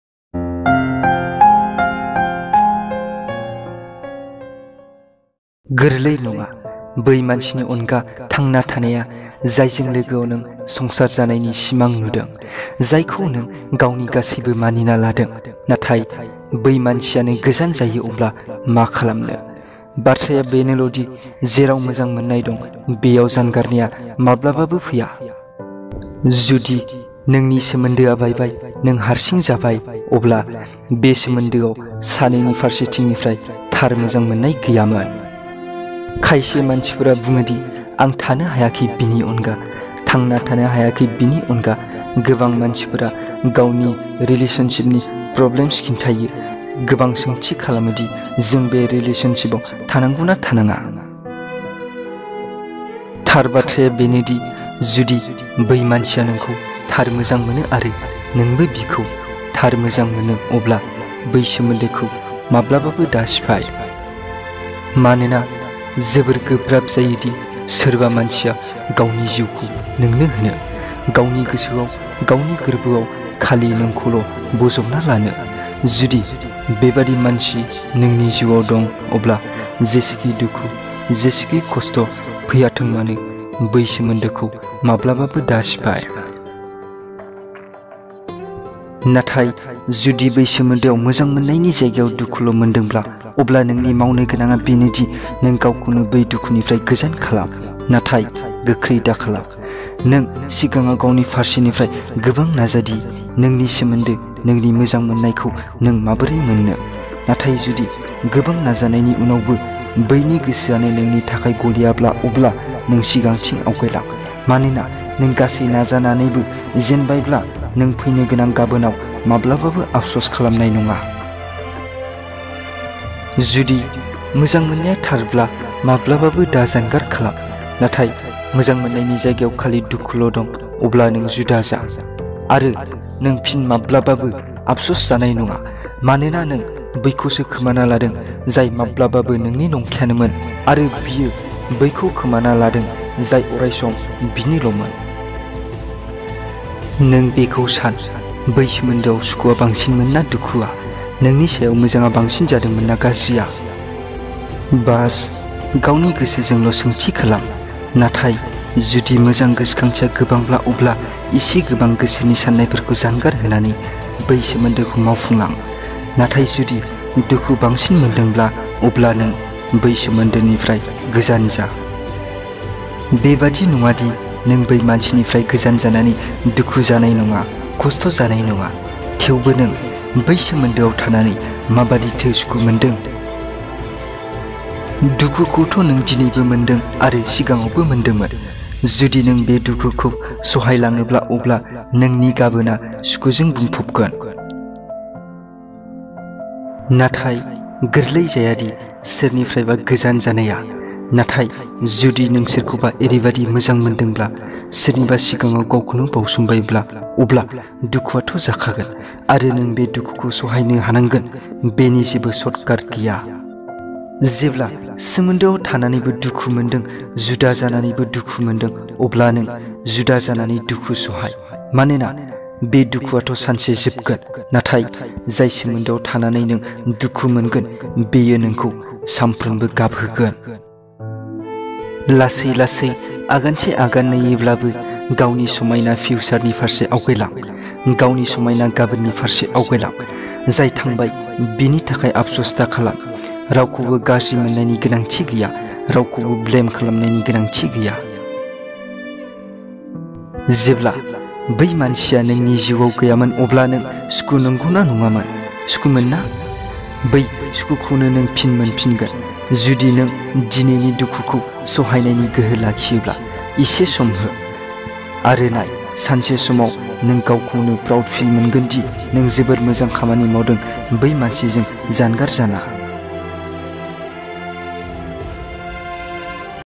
True love bodo quotes| Heart touching bodo love shayari|Bodo Motivation video
True love bodo motivation video|Heart touching bodo love shayari|Bodo motivation video

बे video आव आं थार मोजां मोननाय नि सोमोन्दै बुंदों। मानोना जेराव थार मोजां मोननाय दं बेयाव जानगार लायनाय आ माब्लाबाबो फैआ...आरो जुदि नोंसोर सानैजों जुदा जाबाय अब्ला सानैनि गेजेराव थार मोजां मोननाय गैयामोन। मानोना थार बाथ्राया बेनोदि.. जुदि बै मानसिआ नोंखौ थार मोजां मोनो आरो नोंबो बिखौ थार मोजां मोनो अब्ला बै सोमोन्दोखौ माब्लाबाबो दा सिफाइ. मानोना जोबोर गोब्राब जायो सोरबा मानसिआ गावनि जिउ नोंनो होनो, गावनि गोसो गावनि गोरबोआव खालि नोंखौल'बजबना लानो...
     जुदि नोंनि जिउ आव बेबादि मानसि दंब्ला बै सोमोन्दोखौ माब्लाबाबो दा सिफाइ...
       I hope you are all enjoyed the video..🥰😍